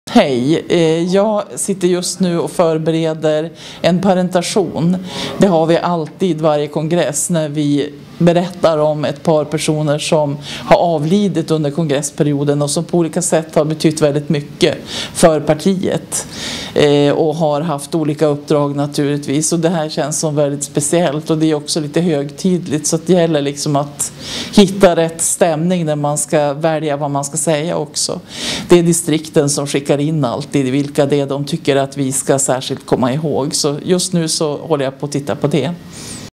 Anki Ahlsten partisekreterare
Anki Ahlsten partisekreterare berättar lite om vad hon gör från Vänsterpartiets kongress 2010